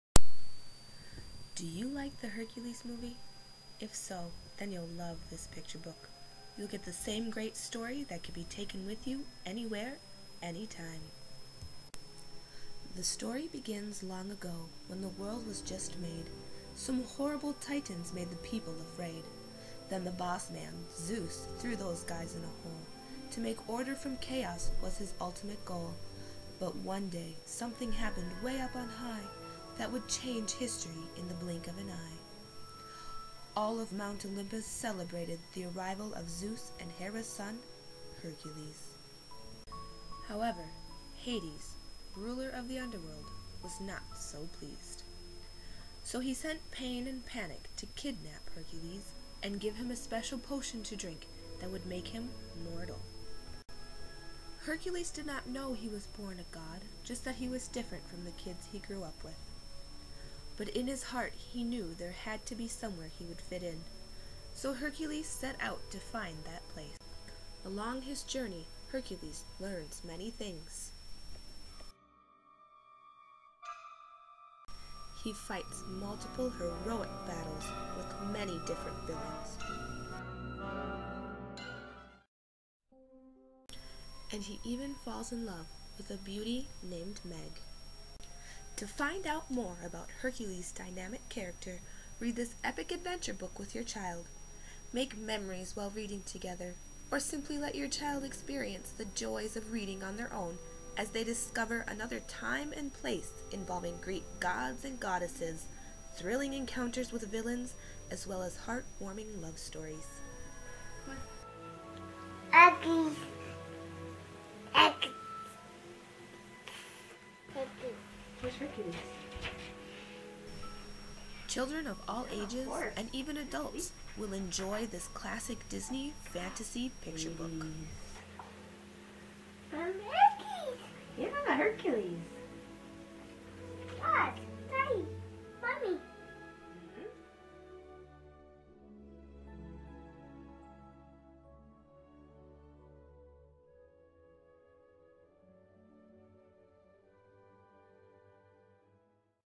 hercules picturebook trailer
Video created for a Children's Literature college course at the U of M.